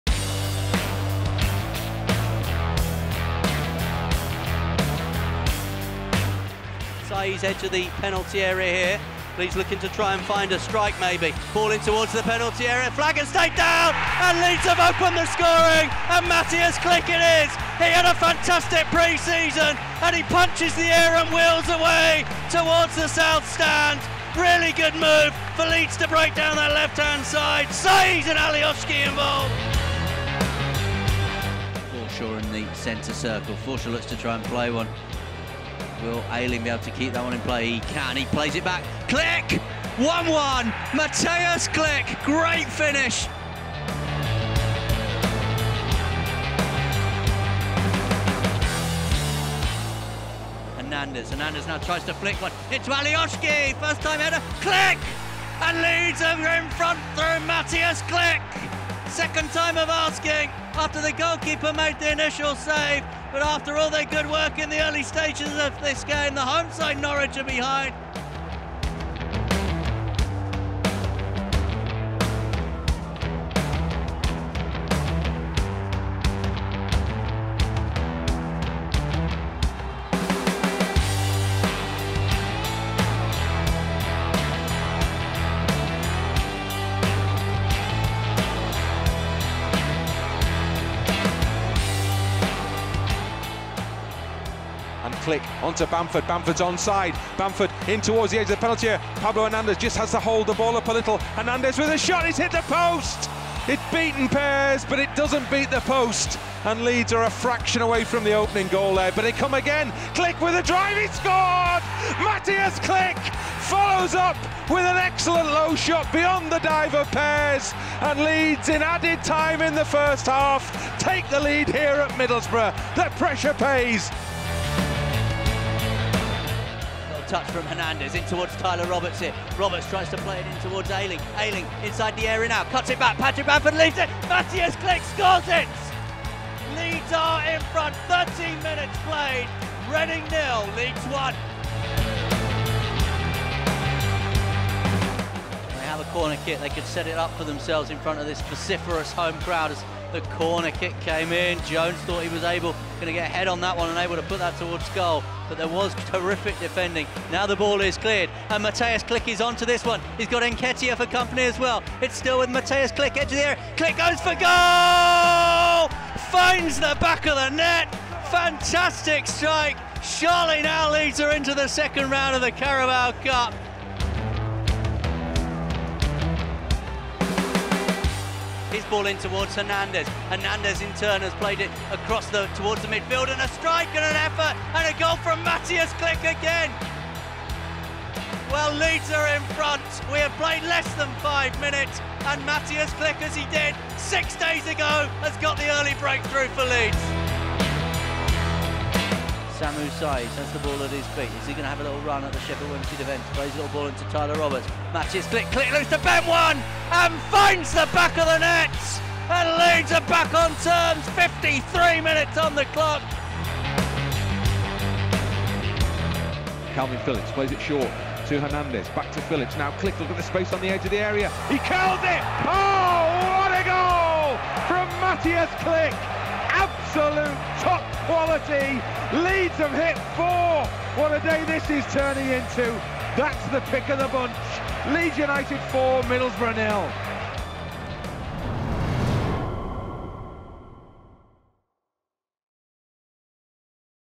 Top 10 goals: Mateusz Klich | Leeds United
Watch our countdown of Mateusz Klich’s top 10 goals for Leeds United.